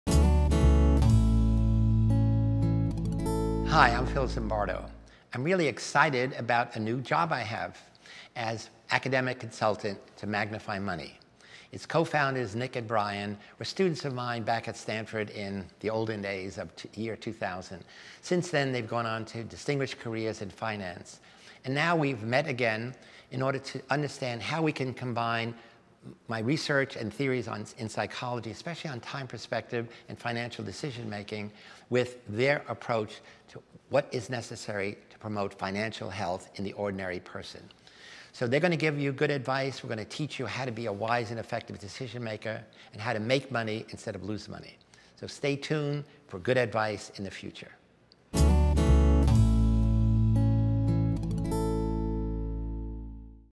Professor Philip Zimbardo's role with MagnifyMoney.com
Professor Philip Zimbardo discusses his role as academic consultant to MagnifyMoney.com